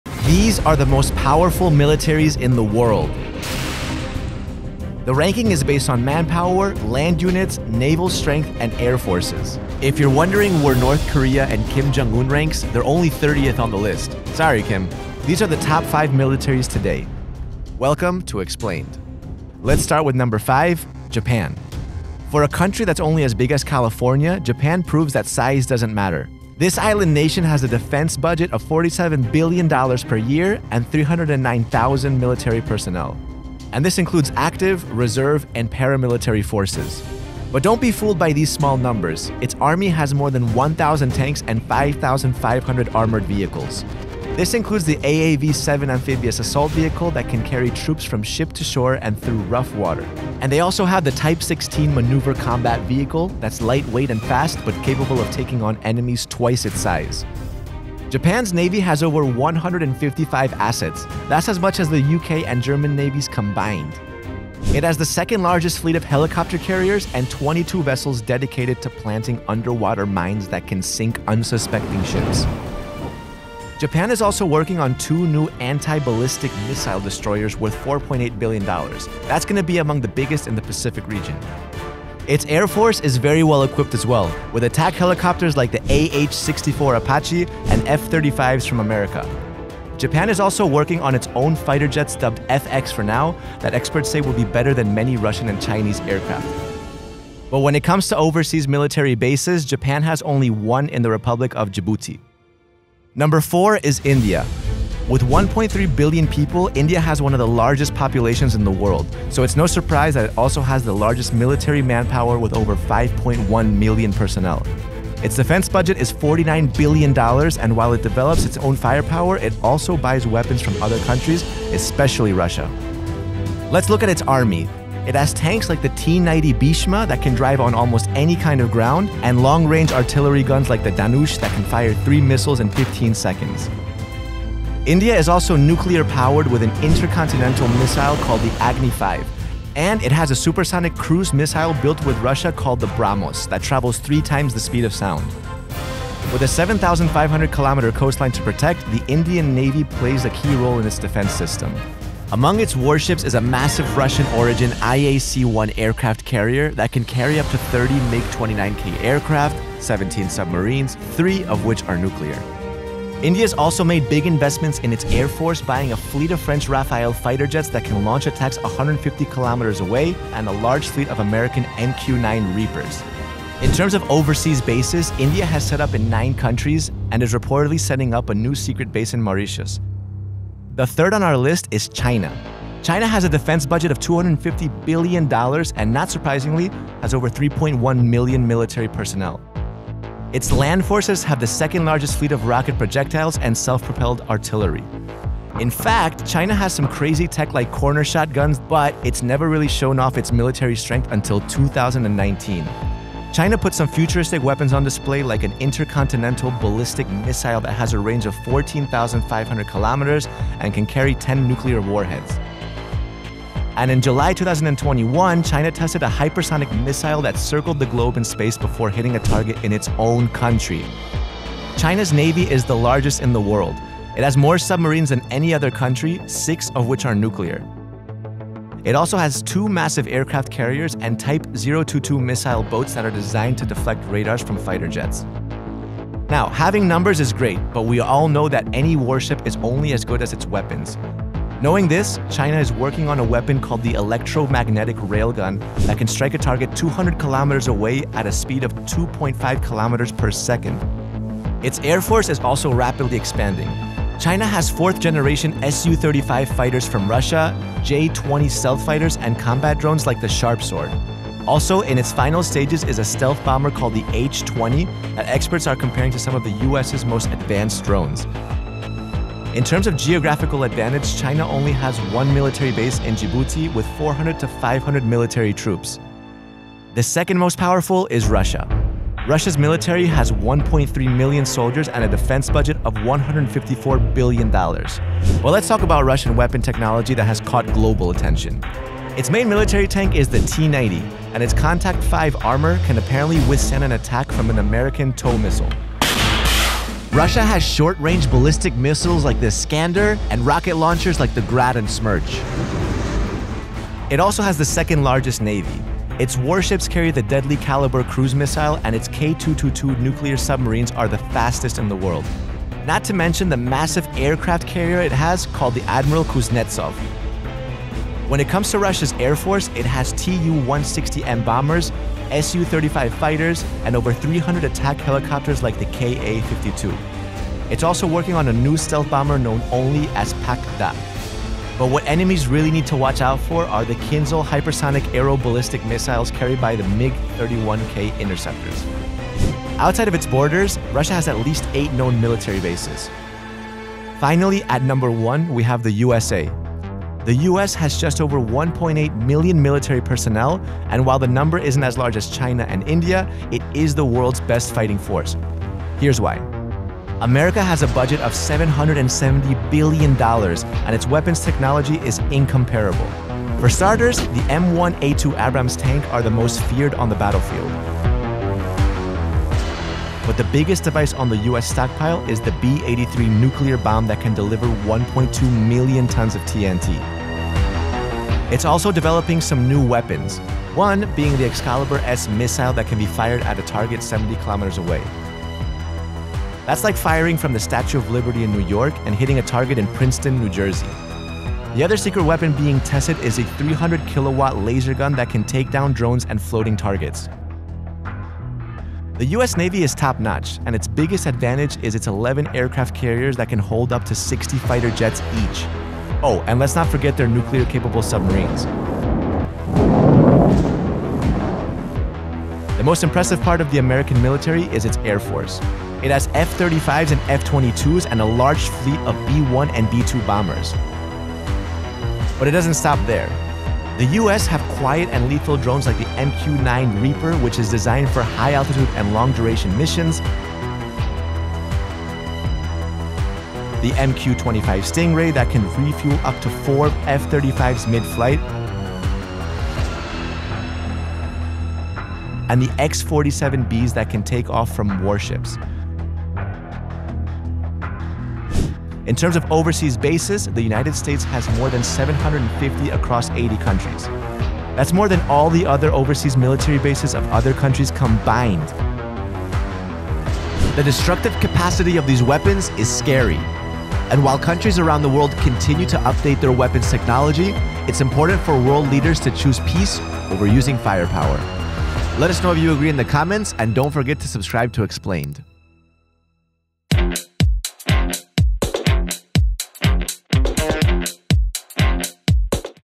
World’s Most Powerful Militaries
Countries all over the world spend billions of dollars on their defense budgets each year in order to position themselves as one of the most powerful nations in the world. Some countries focus much more on this spend than others and in this episode of Xplained we take a closer look at the top 5 most powerful militaries in the world.

Check out the links below for credit to the awesome footage used!